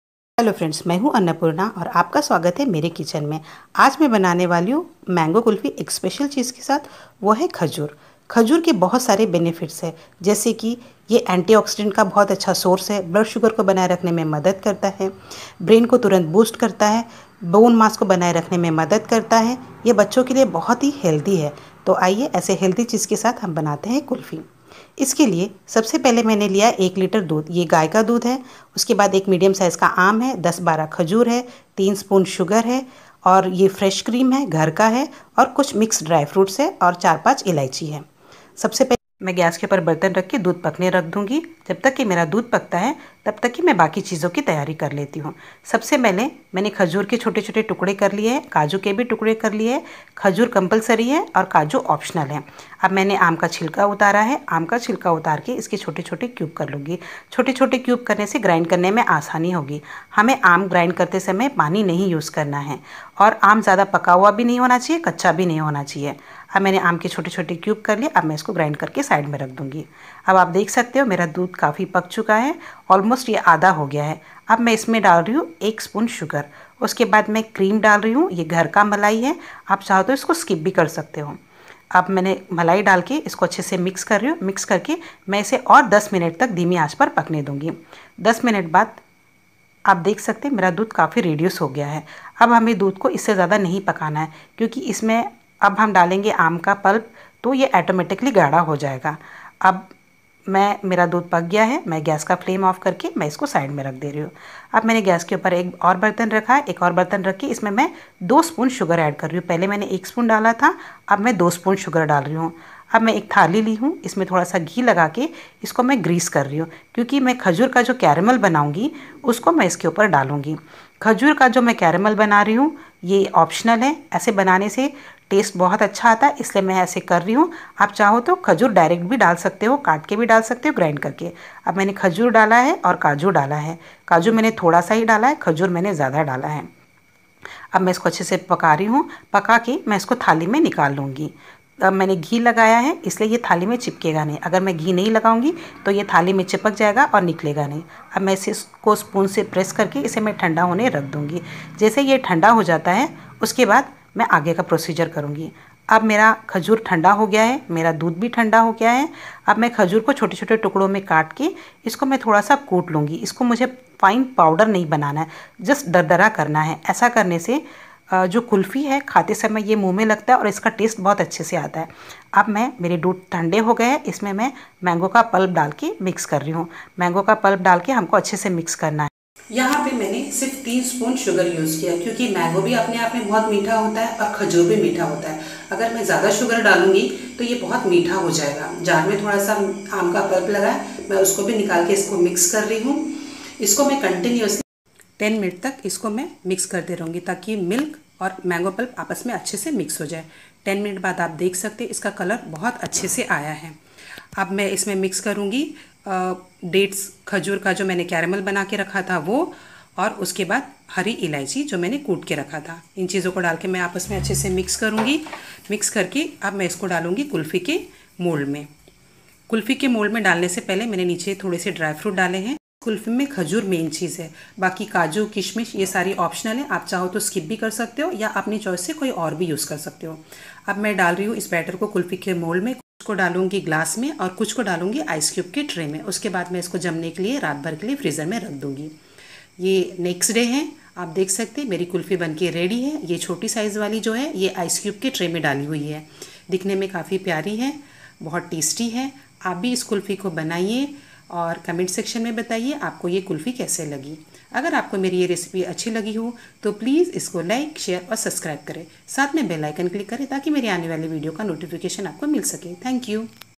Mango Kulfi With Dates Caramel.आम और खजूर की कुल्फी .
Hello freinds today I share kulfi with mango and dates.
In this recipe I use mango and dates.
Mango “The king of the fruits," mango fruit is one of the most popular, nutritionally rich fruits with unique flavor, fragrance, taste, and heath promoting qualities and Dates are a source of antioxidants, good for blood sugar balance, reduce blood pressure.
And Dates contain a brain boost & helps in maintain bone mass.